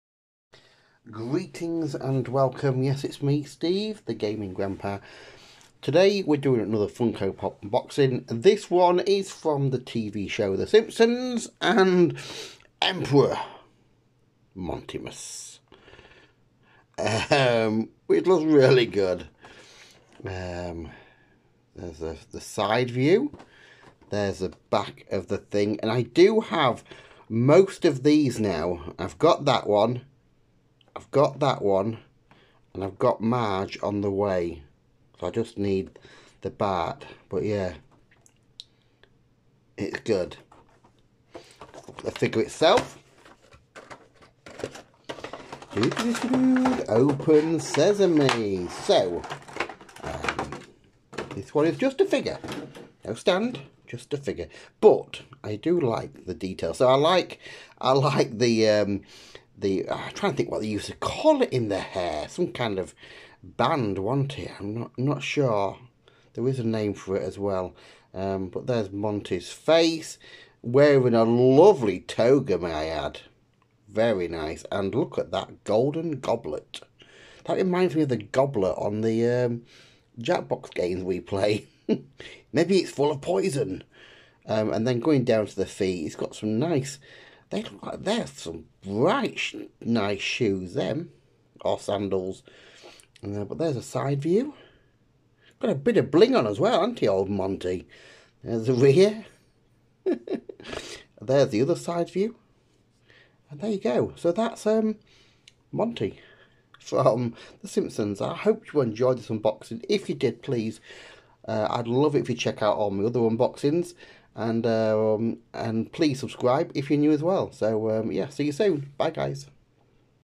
Funko Pop Unboxing - The Simpsons Emperor Montimus
this is emperor montimus from the simpsons range.